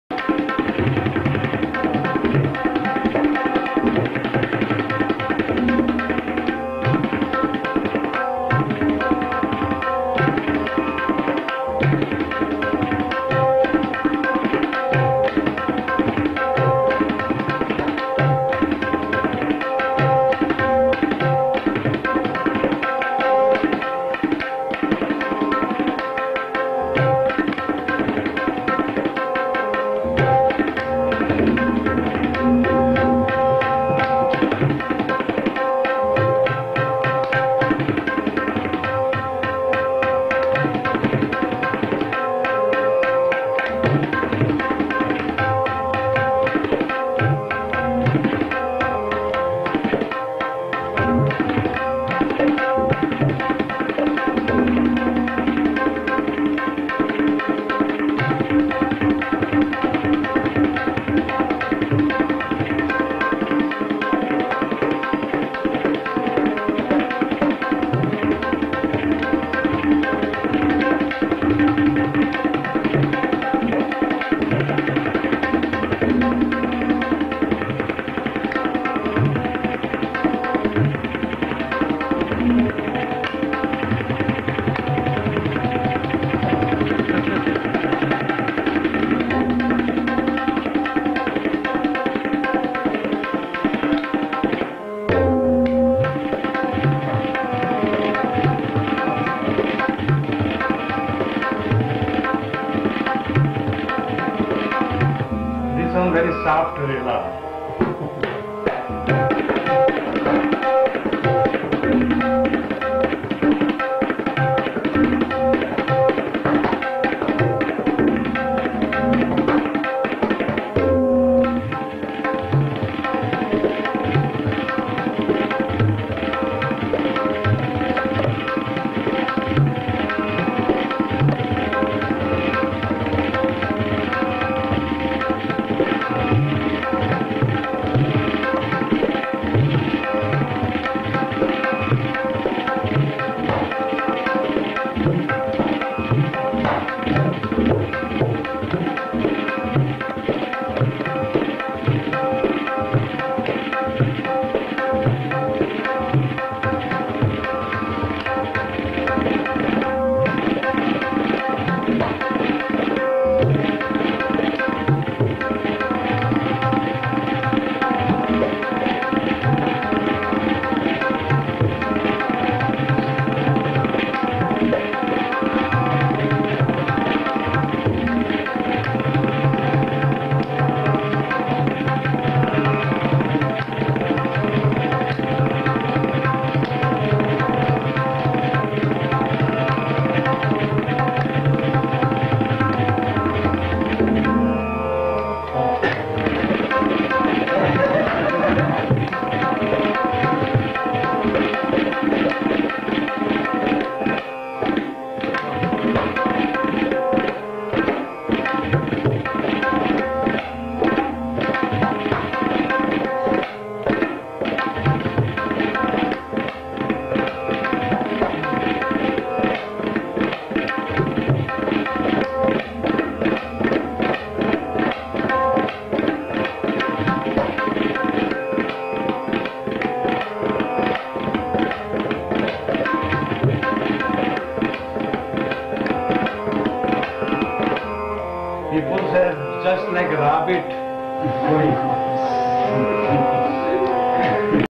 Artistry of Pandit Sharda Sahai
Here we see Pt Sharda Sahai display his masterful and graceful touch on the Tabla to delighted students, disciples and guests in North America.

For over 25 years PRSSV ran Tabla camps across North America with students able to dedicate a period  to learn  in a residency and  absorb the musical culture.